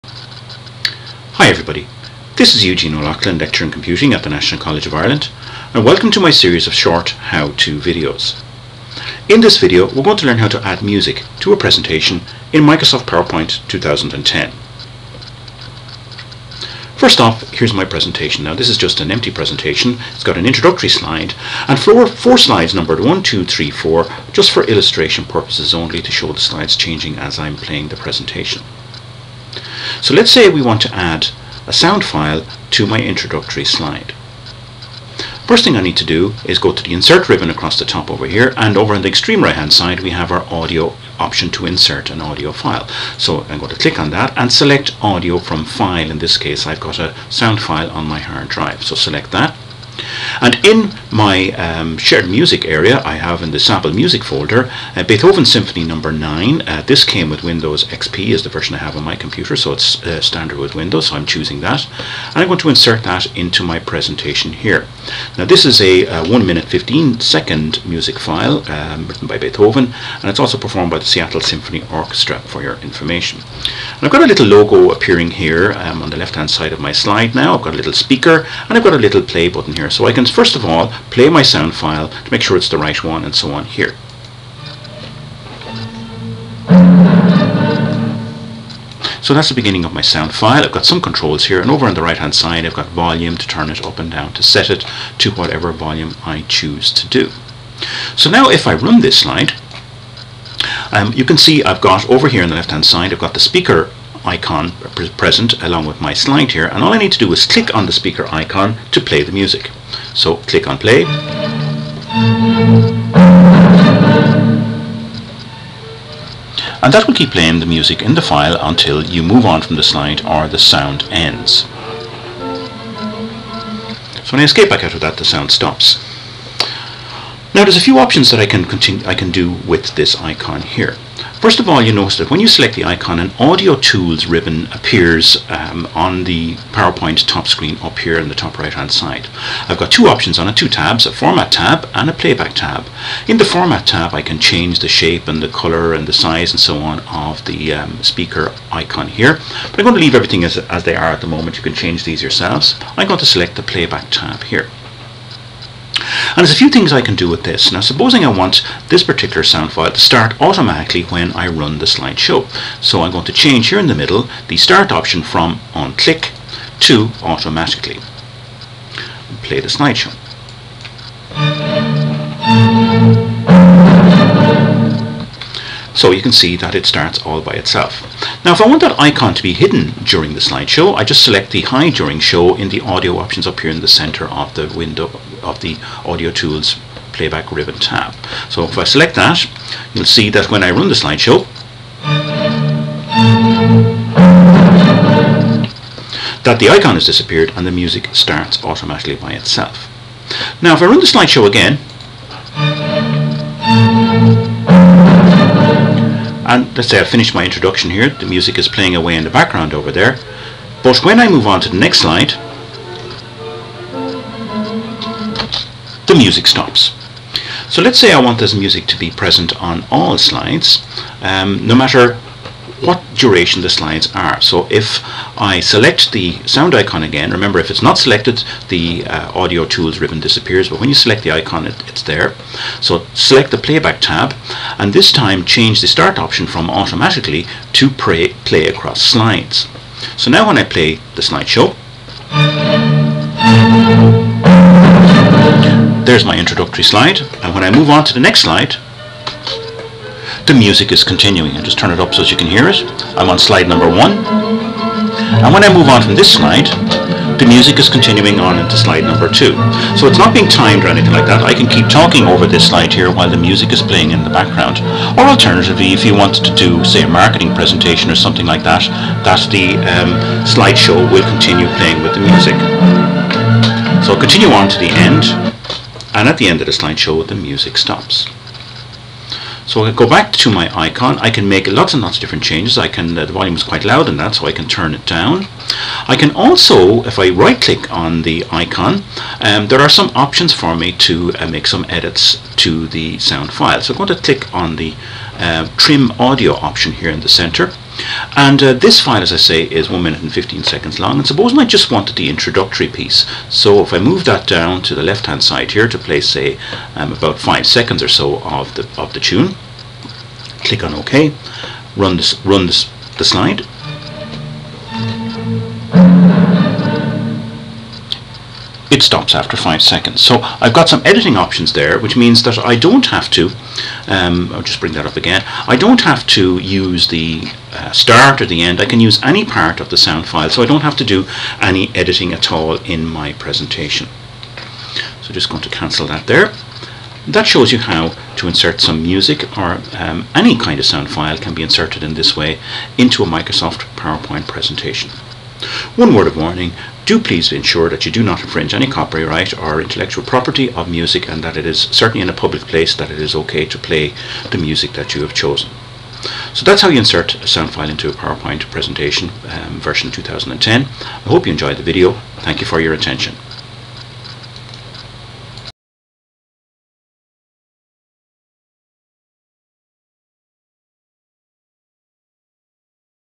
How To... Add Music to a Presentation in PowerPoint 2010
Learn how to add a music audio file to a PowerPoint 2010 presentation in simple steps. You can play an audio file for one slide, or over several slides as you wish.